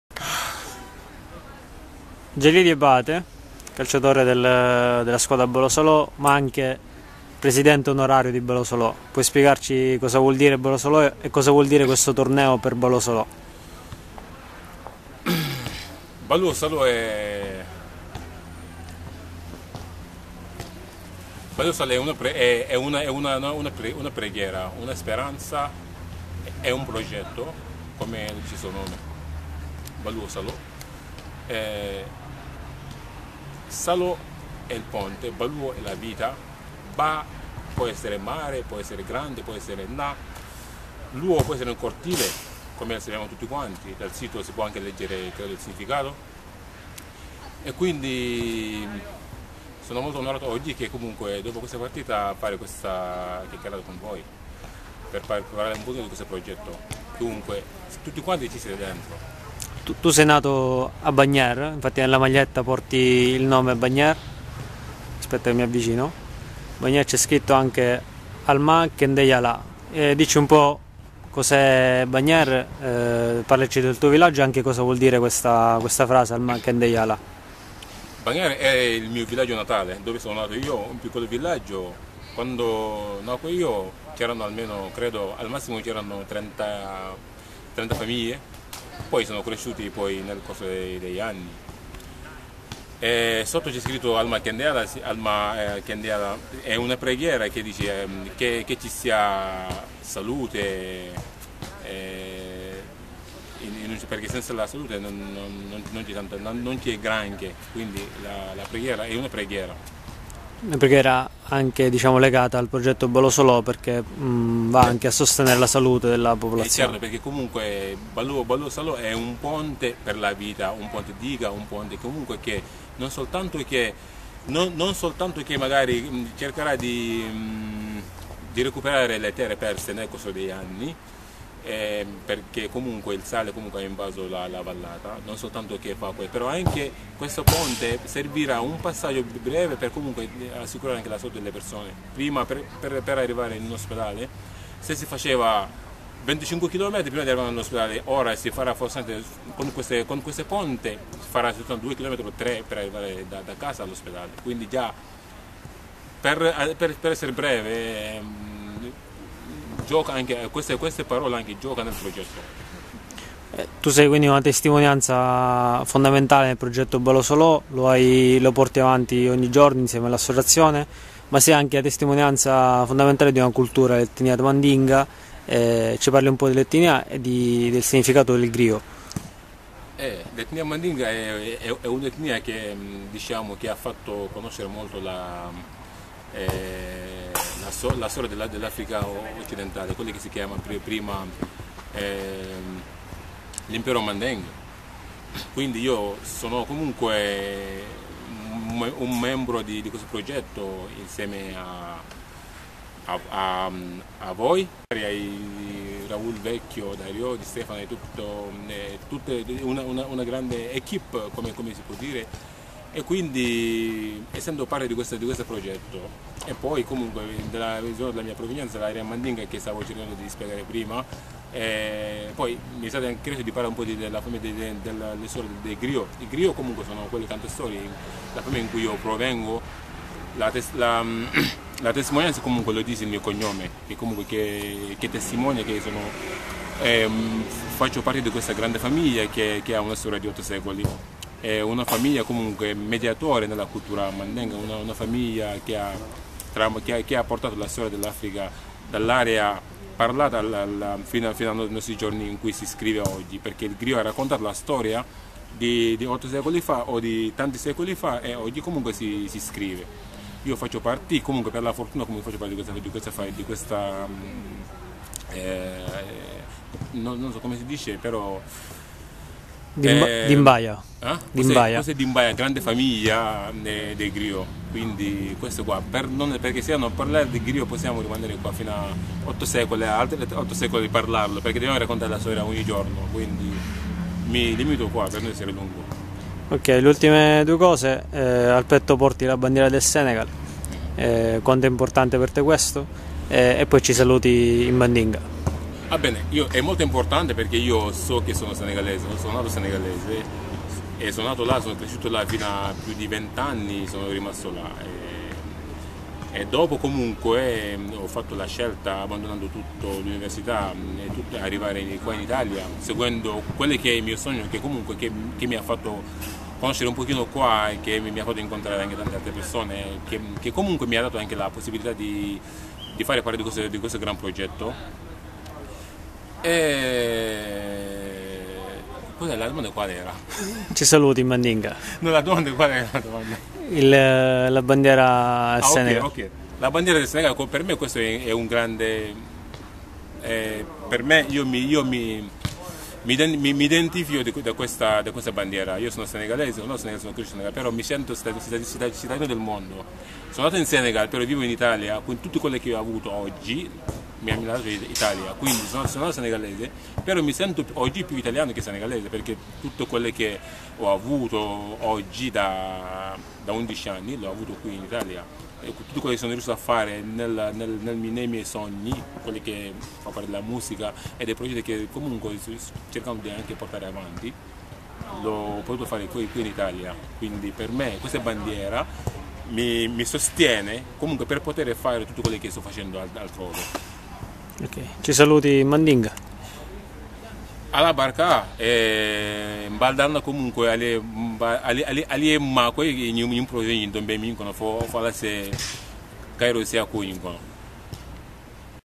Intervista a Jali Diabate, presidente onorario della Onlus Balouo Salo
Jali Diabate è il presidente dell'Associazione Balouo Salo, nonchè testimone delle problematiche sociali della regione Casamance. E' uno dei più importanti cantautori senegalesi in Italia, Senegal e Francia, e testimonia la sua cultura e Balouo Salo nel mondo anche attraverso la musica.
Uno dei 350 villaggi beneficiari del progetto Balouo Salo in Casamance, è il villaggio di Baghere, dove Jali è nato, e che ha lasciato più di 10 anni fa per venire in Italia.

Master Cup è il torneo della solidarietà organizzato per sostenere il progetto Balouo Salo e promuovere le tematiche di integrazione, amicizia e fratellanza tra i popoli. L'Associazione Balouo Salo organizza il torneo e vi partecipa con una squadra di rappresentanza, di cui Jali fa parte. Nel petto i colori della bandiera del Senegal, e il nome della regione in cui il progetto si insedia, sulle spalle le preghiere in lingua mandinga.